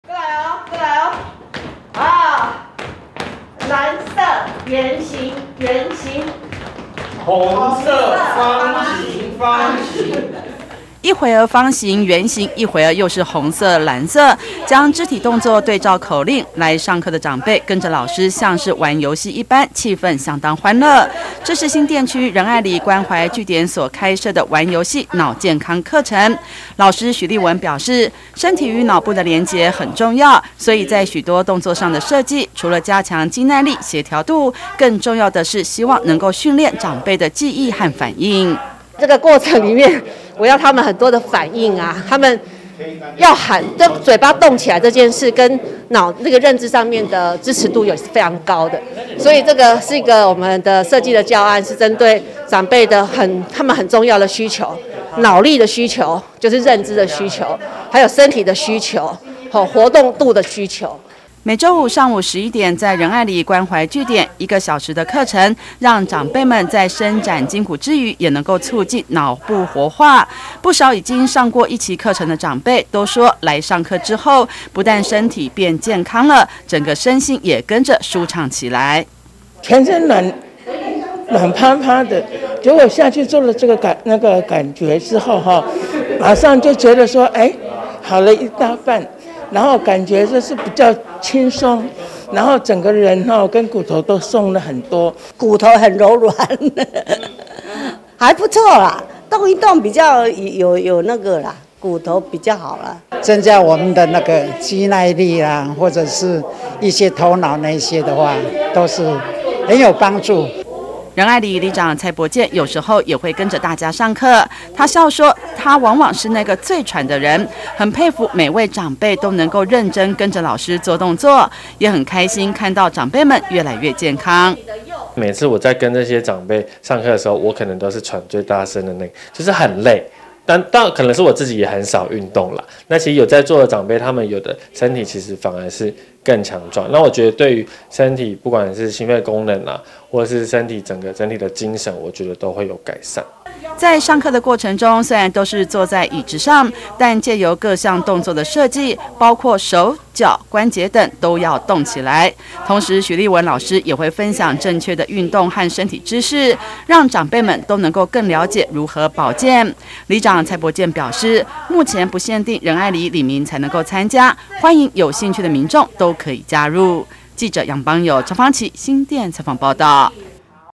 新店仁愛里關懷據點 開設玩遊戲腦健康課程
新店區仁愛里關懷據點開設-玩遊戲 腦健康 課程，藉由各項肢體動作，結合口令，讓長輩像是玩遊戲一樣，不但可以活動筋骨、加強肌耐力，同時還促進腦部活化，訓練記憶力和反應，長輩們都說每次都很期待來上課，讓自己身心舒暢又健康。